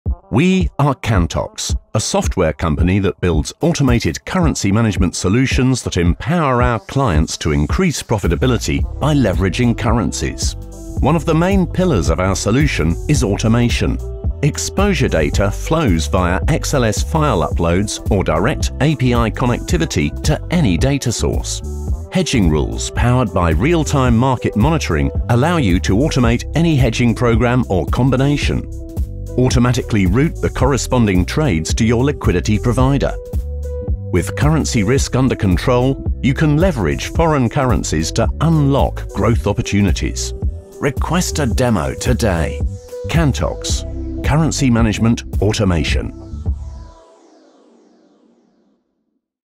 Kantox - Automation
We are Kantox, a company that builds automated currency management solutions that empower clients to increase profitability by leveraging currencies.

One of the main pillars of our solution is automation.

Discover how automation provides direct connectivity to any data source, automate any hedging program or combination, and route corresponding trades to the liquidity provider.